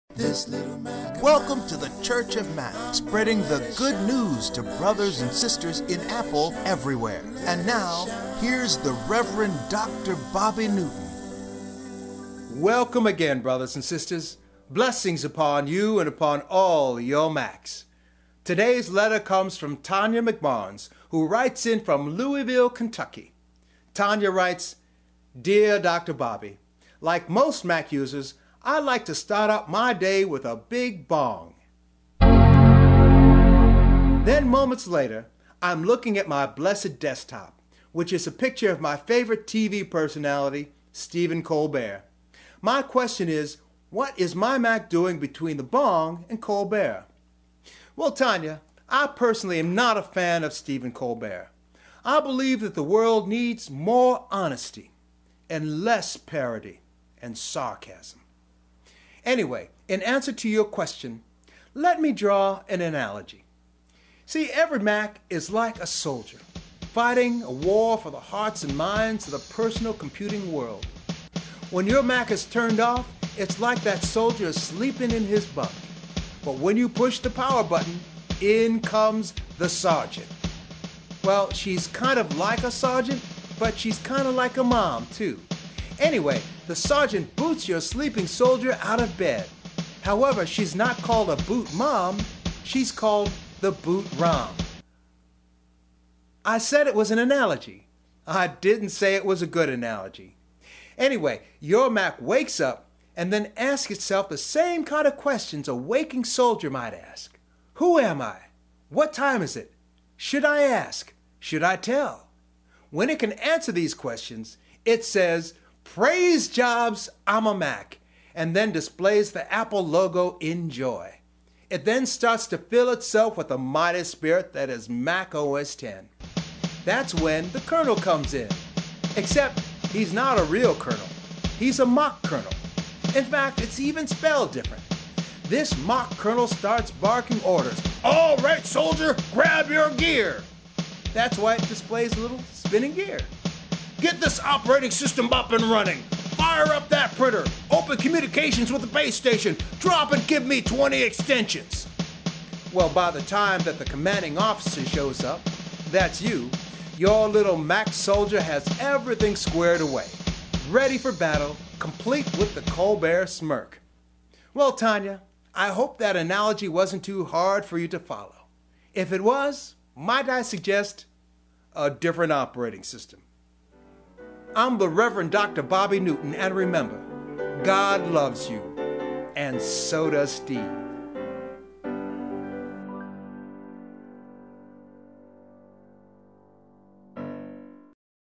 The Little Soldier
Dr. Bobby explains the Mac start-up sequence.